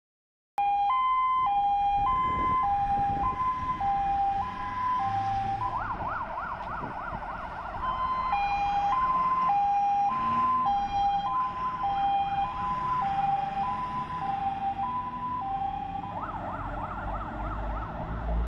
Wyjazd alarmowy do wypadku Mercedes GBA 2,5/24 421[E]21 i SLRt Renault Mascott 421[E]40
Wyjazd 421[E]40 SLRt Renault Mascott 160.65/ISS Wawrzaszek i 421[E]21 GBA 2,5/24 Mercedes Benz Atego 1329F/Moto Truck